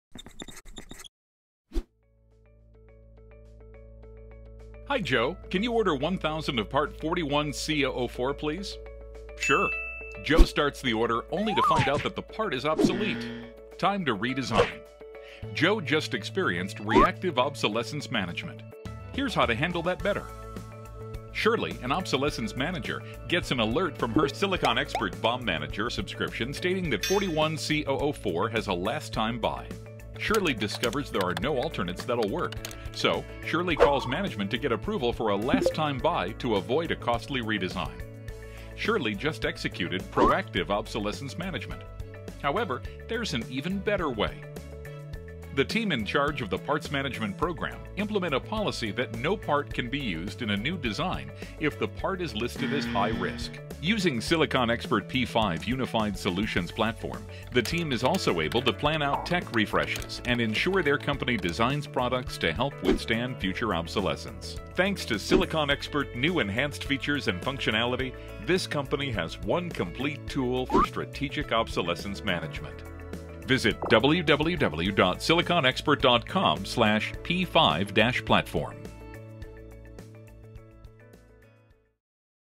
Obsolescence Management Done Right
What is strategic obsolescence management?  End of life forecasting, proactive alerts, mitigating risk within your supply chain.  Learn how SiliconExpert's P5 Platform solutions can assist in strategically managing your supply chain.